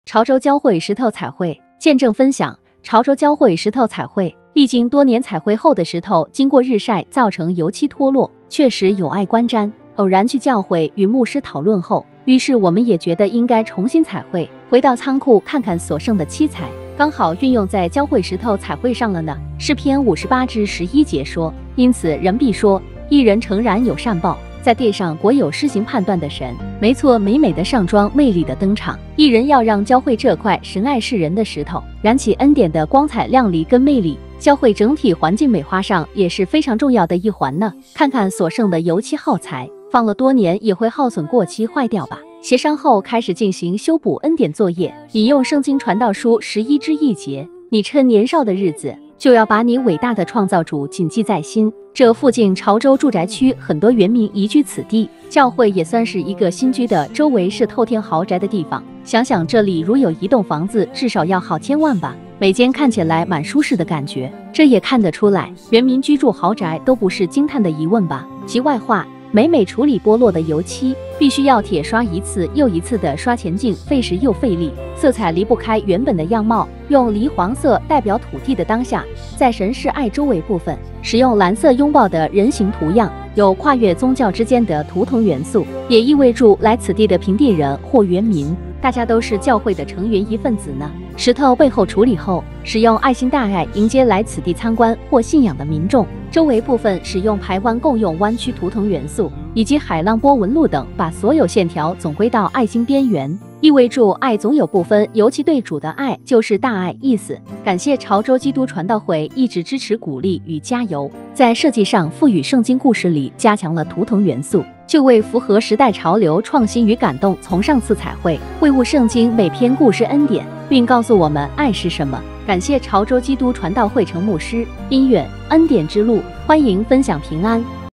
潮州基督傳道會石頭彩繪口述版
潮州教會石頭彩繪
見證分享：

潮州教會石頭彩繪，歷經多年彩繪後的石頭，經過日曬造成油漆脫落，確實有礙觀瞻！偶然去教會與牧師討論後，於是我們也覺得應該重新彩繪，回到倉庫看看所剩的漆材，剛好運用在教會石頭彩繪上了呢。

詩篇58-11節說
因此，人必說：義人誠然有善報；在地上果有施行判斷的神！

沒錯，美美的上妝，魅力的登場！義人讓教會這塊神愛世人的石頭，再有光彩的亮麗跟魅力了呢！

教會整體環境美化上也是非常重要的一環呢！看看所剩的油漆耗材，放了多年也會耗損過期壞掉吧，協商後擇日再進行修補作業。

引用聖經：「你趁年少的日子，就要把你偉大的創造主謹記在心。」（傳道書12:1）

潮州附近很多原民居地，教會也算是一個新居地，週圍是透天豪宅的地方，想想這裡如有一棟房子，至少要好千萬吧？每間看起來滿舒適的感覺。

這也看的出來，原民居住豪宅都不是驚嘆的疑問吧？

題外話，每每處理剝落的油漆，必須要鐵刷一次又一次的刷乾淨，費時又費力！

色彩離不開原本的樣貌，用黎黃色代表土地的當下，在神是愛週圍部分，使用藍色擁抱的人行圖樣！有跨越宗教之間的圖騰元素，也意味著來此地的平地人或原民大家都是教會的成員一份子呢！

石頭背後處理後，使用愛心大愛迎接來此地參觀或信仰的民眾。

週圍部分使用排灣共用彎曲圖騰元素，以及海浪波紋路等，把所有線條總歸到愛心邊緣，意味著愛總有不分，尤其對主的愛就是大愛意思。

感謝潮州基督傳道會一直支持鼓勵與加油！在設計上賦予聖經故事裡加強了圖騰元素，就為符合時代潮流創新與感動，也從上次彩繪中，會晤聖經每一篇故事，告訴我們的愛是什麼？要怎麼順服跟鼓勵他人。

平安